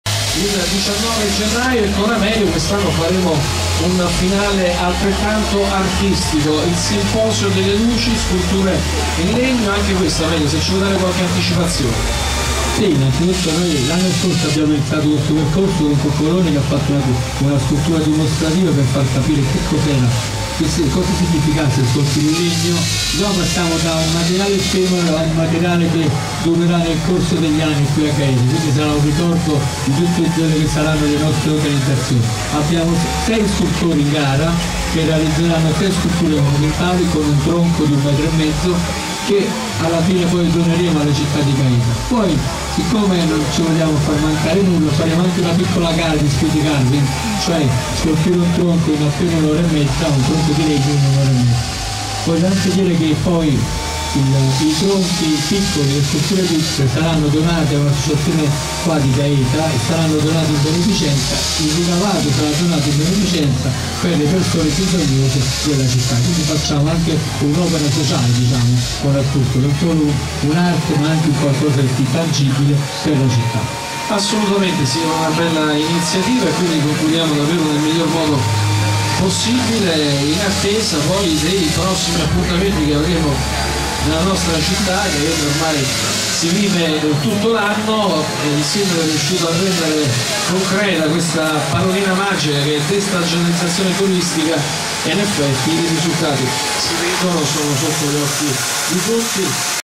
SIMPOSIO DELLE LUCI: 1° CONCORSO DELLE SCULTURE DI LEGNO
SIMPOSIO DELLE LUCI WOOD CARVING FESTIVAL
1° CONCORSO DELLE SCULTURE DI LEGNO

GAETA ~ VIALE BATTAGLIONE DEGLI ALPINI (VIALE ALBERATO) 16 ~ 19 GENNAIO 2020 ALLE ORE 16.00

INTERVISTA AL PRESIDENTE DELL' ASSOCIAZIONE ITALIANA SCULTURE DI GHIACCIO, AMELIO MAZZELLA A CURA DI ANDREA BRENGOLA

I PARTECIPANTI:

🇮🇹 Roberto Coccoloni - Firenze
🇮🇹 Francesco Battaglini - Firenze
🇮🇹 Ferdinando Gatto - Rogliano (CS)
🇮🇹 Michelle Consalvo - Capurso (Bari)
🇮🇹 Maurizio Ferrari - Verona
🇮🇹 Silvio Viola - Pistoia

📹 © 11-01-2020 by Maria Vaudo